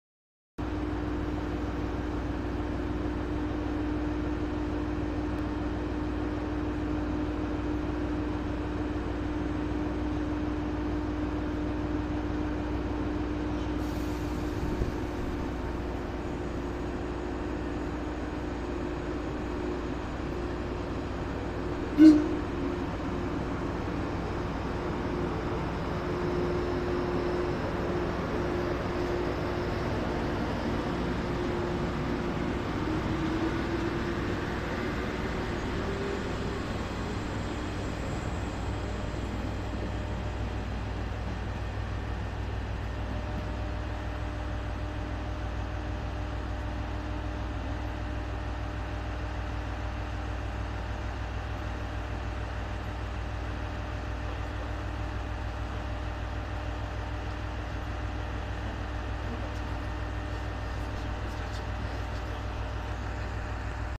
急行2号直江津行き(クハ455-701+モハ412-6+クモハ413-6) 糸魚川発車
急行2号直江津行き(クハ455-701+モハ412-6+クモハ413-6)　糸魚川発車です。